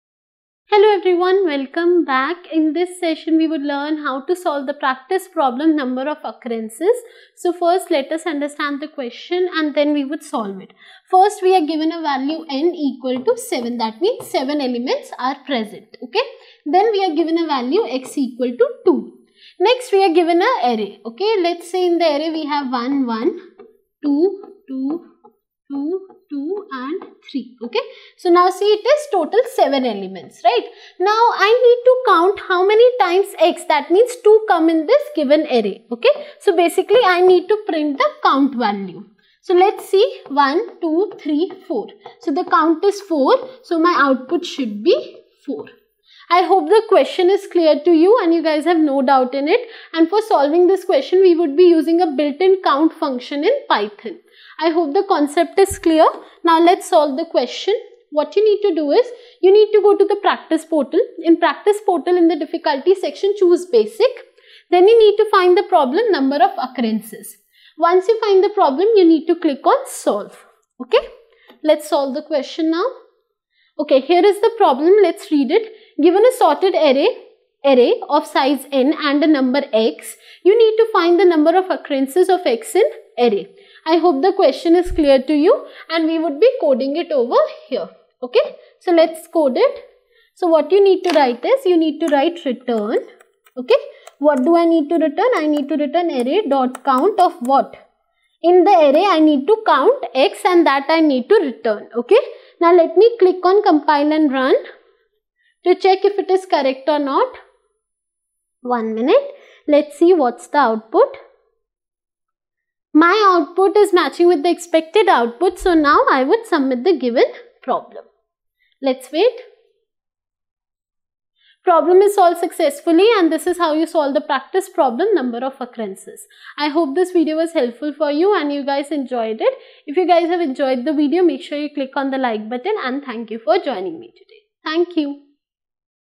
Number of occurrence | School Practice Problem | GeeksforGeeks School
Join Avneet Kaur as she solves the school practice problem: Number of occurrence. This is a great way to improve your coding skills and analyze yourself.